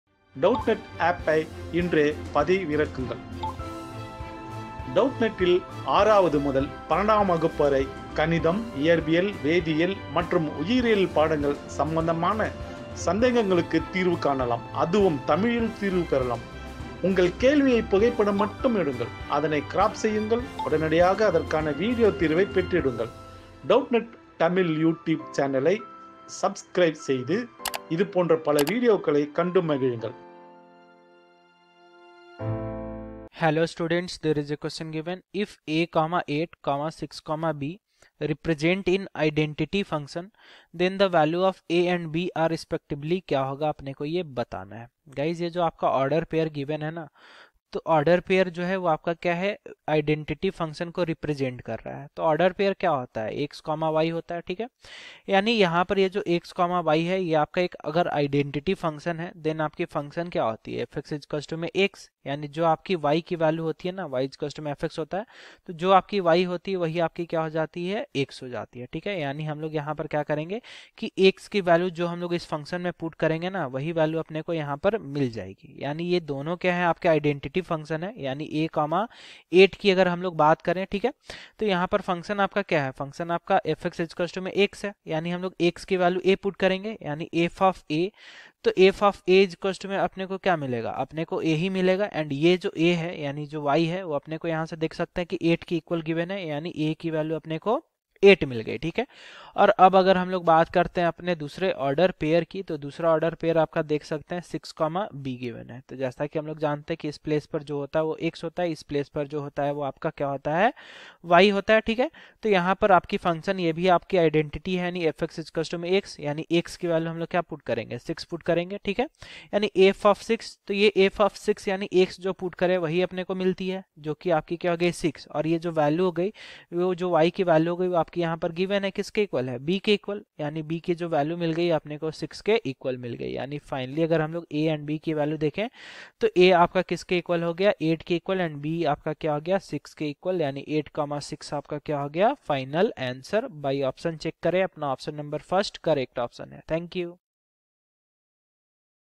If {(a,8),(6,b)} represent in identity function, then the value of | Class 10 Maths | Doubtnut
If {(a,8),(6,b)} represent in identity function, then the value of a and b are respectively .

Online Class में Admission लेने के लिए यहाँ click करें :
👉 Class 10 | CBSE Boards | English Medium :

👉 Have Any Query? Ask Us.